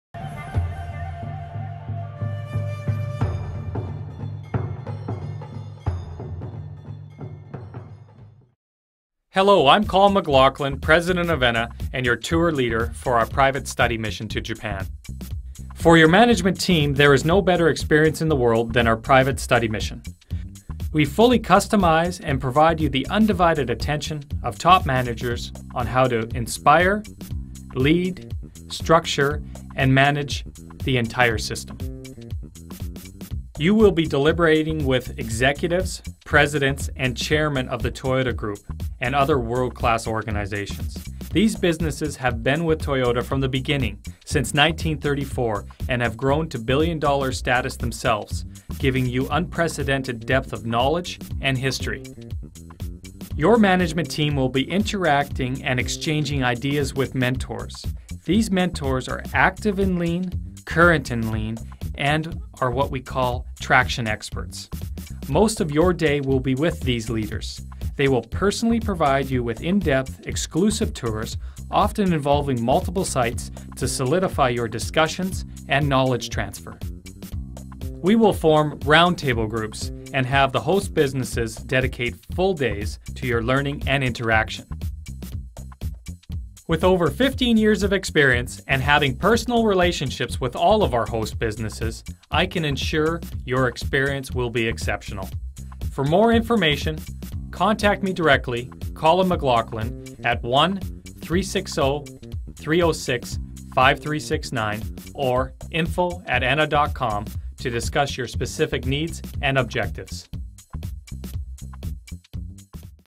Private Study Mission to Japan with ENNA
We offer you a full customization of Enna's Japan tours

We know that tour participants work in a multitude of industries, so we are constantly working with new businesses to offer you a greater selection of industry focus; whether supply chain, retail, marketing, government, manufacturing, office, healthcare, we look for the best benefit to you. This also allows us to customize your tour based upon specific Lean tools that you want to learn more about. Personal interactions with both shop floor workers and management provides exclusive, individualized learning tailored to your needs.